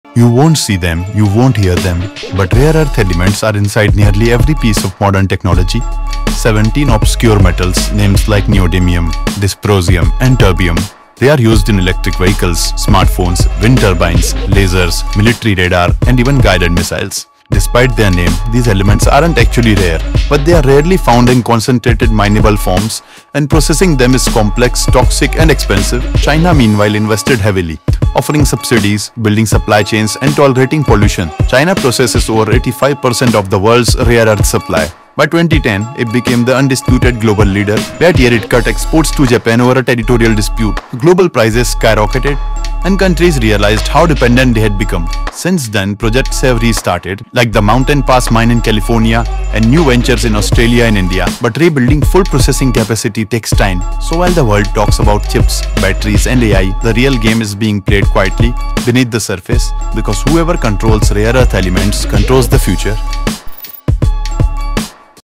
The Hidden Metals That Control the Future (And Who Owns Them)
Rare earth elements are in every phone, EV, and missile system — and nearly all of them come from one country. Here’s why that’s a big deal.